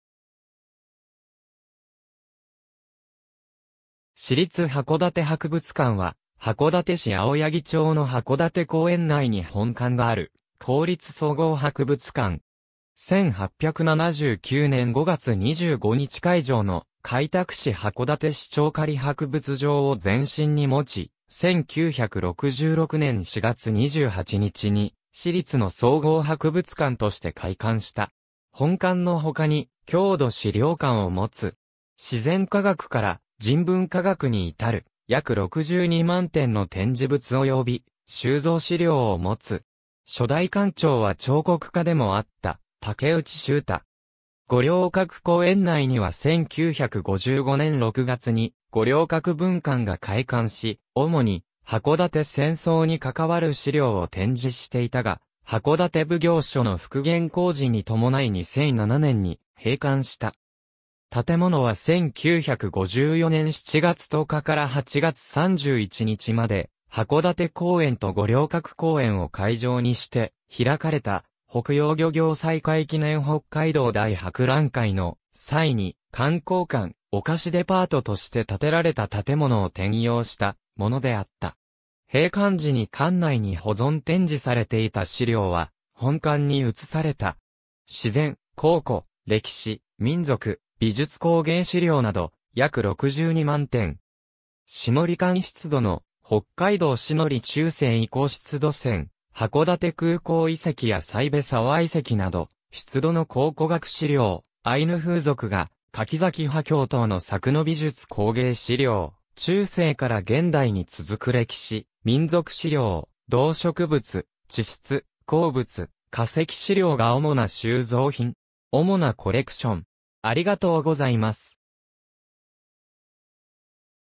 市立函館博物館
市立函館博物館（しりつはこだてはくぶつかん）は、函館市青柳町の函館公園内に本館がある公立総合博物館。
1879年5月25日開場の開拓使函館支庁仮博物場を前身に持ち、1966年4月28日に市立の総合博物館として開館した。
本館の他に郷土資料館（旧金森洋物店、1969年11月開館）を持つ。
自然科学から人文科学にいたる、約62万点の展示物および収蔵資料を持つ。
初代館長は彫刻家でもあった竹内収太。
五稜郭公園内には1955年6月に五稜郭分館が開館し、主に箱館戦争に関わる資料を展示していたが、箱館奉行所の復元工事に伴い2007年に閉館した。
建物は1954年7月10日から8月31日まで函館公園と五稜郭公園を会場にして開かれた北洋漁業再開記念北海道大博覧会の際に「観光館・お菓子デパート」として建てられた建物を転用したものであった。
閉館時に館内に保存展示されていた資料は本館に移された。
自然・考古・歴史・民俗・美術工芸資料など約62万点。
志苔館出土の「北海道志海苔中世遺構出土銭」（重要文化財）、函館空港遺跡やサイベ沢遺跡など出土の考古学資料、アイヌ風俗画、蠣崎波響等の作の美術工芸資料、中世から現代に続く歴史、民俗資料、動植物、地質、鉱物、化石資料が主な収蔵品。
主なコレクション